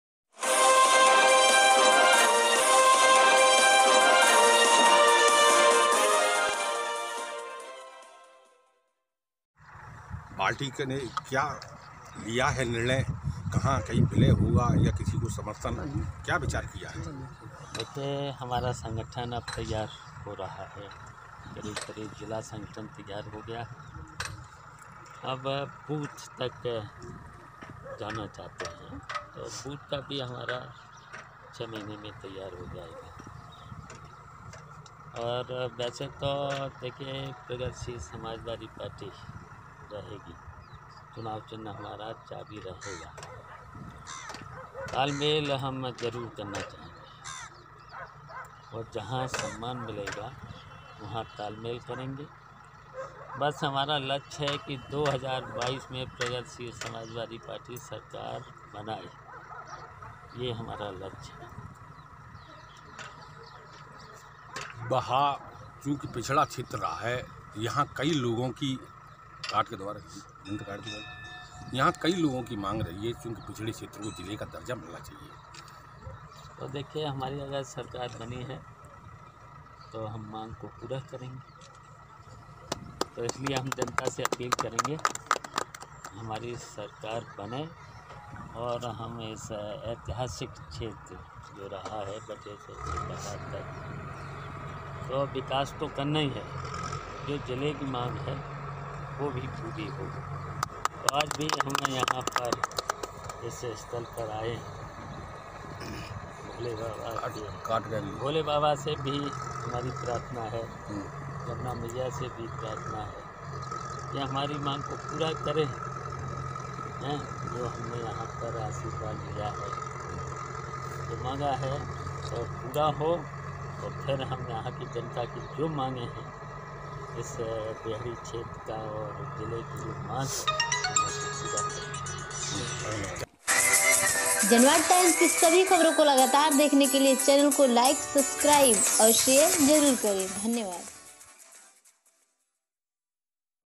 प्रगतिशील समाजवादी पार्टी अध्यक्ष ने बाह को जिला बनाने का किया वादा
Janvad Times :
तीर्थ धाम बटेश्वर के बी एस शिक्षा निकेतन पब्लिक स्कूल  खाद बटेश्वर में स्कूल के वार्षिक सांस्कृतिक रंगा- रंग कार्यक्रम का उदघाटन समाजवादी प्रगतिशील  पार्टी के अध्यक्ष श्री शिवपाल सिंह यादव ने फीता काट कर हरिश्चंद्र नाटक का किया उद्धघाटन।इस मौके पर स्कूल प्रबन्धक श्री मुन्नेश यादव ने साफा बांध कर स्वागत किया । इस मौके और सामाजवादी प्रगति शील के प्रदेश अध्यक्ष नितिन कोहली पूर्व जिला पंचायत अध्यक्ष छोटू यादव उर्फ विजय प्रताप,  महिला प्रकोष्ठ अध्यक्ष मनोरमा, विधान सभा अध्यक्ष राज कुमार फौजी , धर्मेंद्र यादव, मुवीव खान,  राजेश जादौन जसवंत यादव, सुभाष यादव,धारा सिंह,  जिला अध्यक्ष अनीता सिंह प्रदेश अध्यक्ष द्वारका  यादव सहित सभी मौजूद रहे। स्कूल के प्रिंसिपल  दीपसी भदौरिया, तनु सना, फिजा हिना,  निधि , जितेंद्र,  मनोज ,प्रवीन दीपेंद्र सहित सभी स्टाप मौजूद रहे। प्रगतिशील समाजवादी पार्टी अध्यक्ष